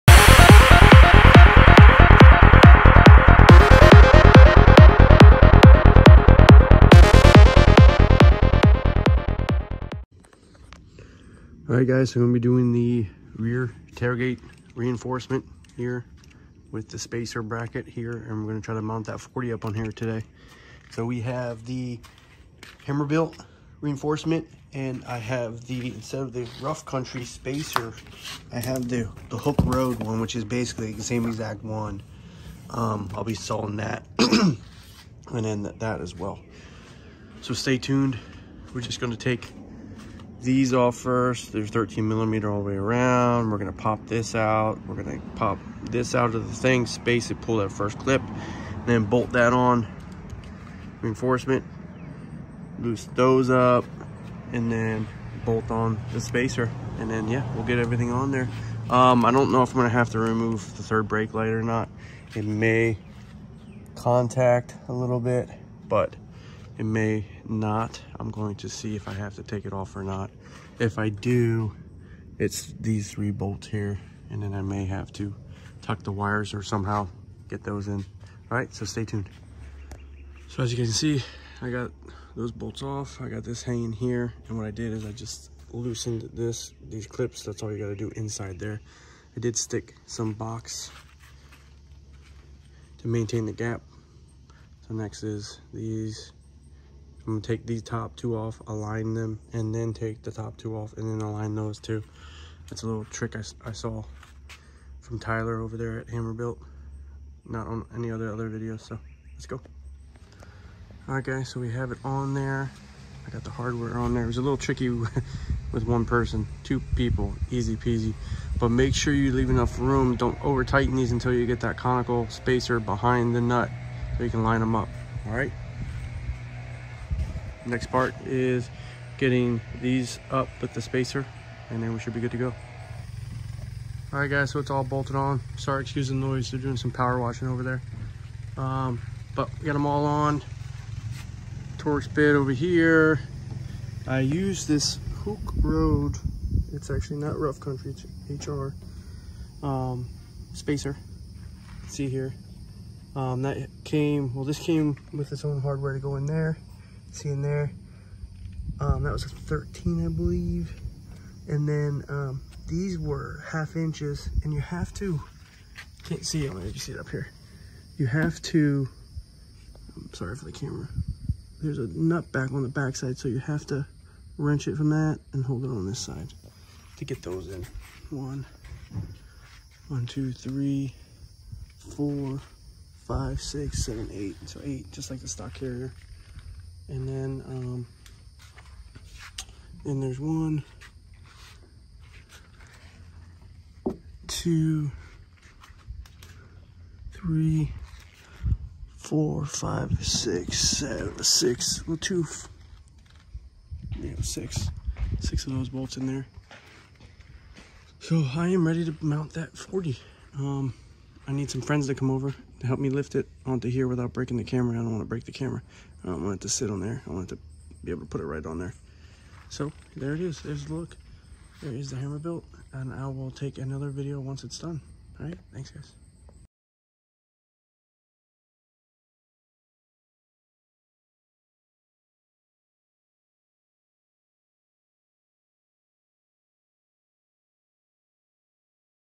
2021 Ford Bronco Hammer Built Tailgate Reinforcement Install
Looking to beef up your rear tail gate to install a 35 or 37 bigger spare tire? We install the Hammer Built rear tire reinforcement bracket on BUCKED to support the massive 40 inch tire spare. We also install a Hooke Road spacer to get it up a bit off of the bumper.

We install all of these mods with our own money.